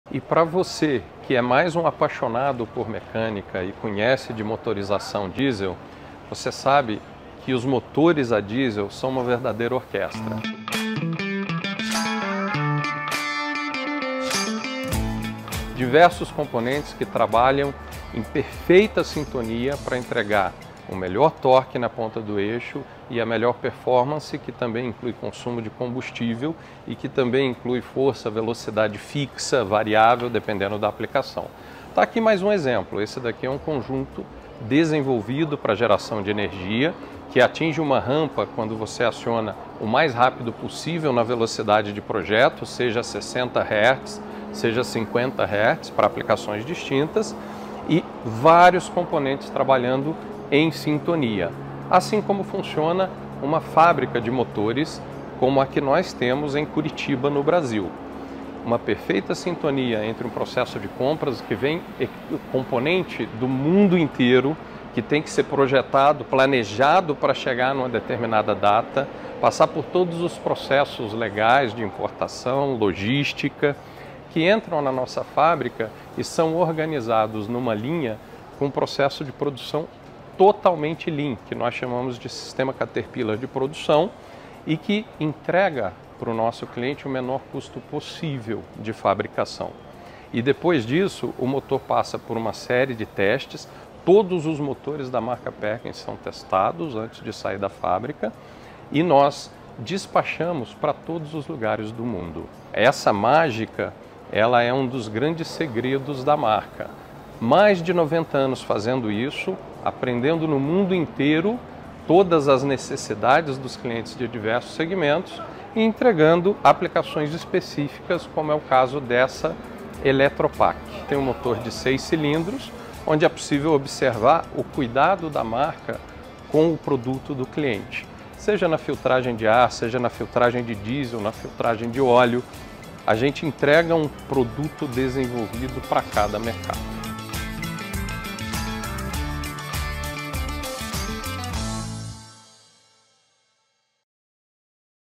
Universo Perkins EP04
Un motor es como una orquesta: varios componentes trabajando en perfecta armonía para proporcionar el mejor torque, consumo de combustible optimizado, potencia y velocidad. Como ejemplo, tenemos un conjunto desarrollado para la generación de energía que alcanza la velocidad de diseño lo más rápido posible para varias aplicaciones diferentes.
La Fábrica de Motores Perkins en Curitiba refleja esta sinfonía. Todo está diseñado y planificado con precisión, desde la importación de componentes globales hasta el proceso de producción ajustada. Y, por supuesto, todos nuestros motores se someten a pruebas rigurosas antes de enviarse a todo el mundo. Este es uno de nuestros secretos para mantener más de 90 años de excelencia, siempre aprendiendo y adaptándonos a las necesidades de los clientes en los diferentes segmentos.